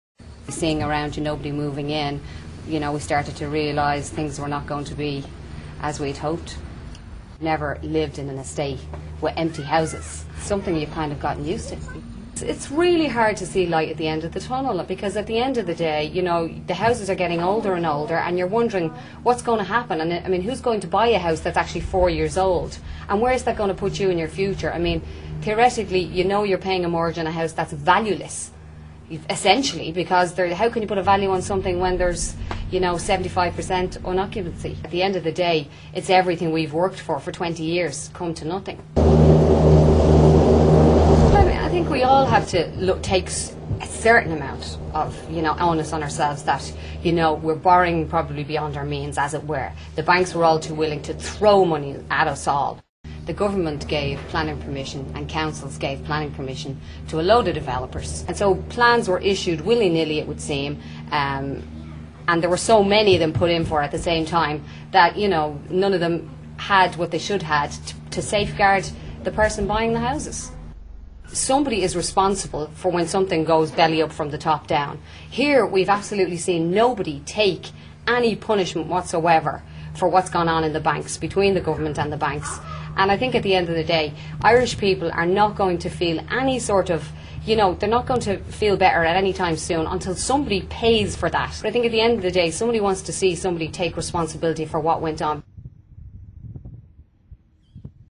Ghost estate houseowner in Ireland.wmv
A ghost estate resident berates the bankers ,the planners, and the politicians who have left her alone and abandoned in one of the hundreds of isolated ,rural  negative equity "Ghost estates"  in Ireland ;far removed from schools, neighbours,shops,hospitals and public transport.!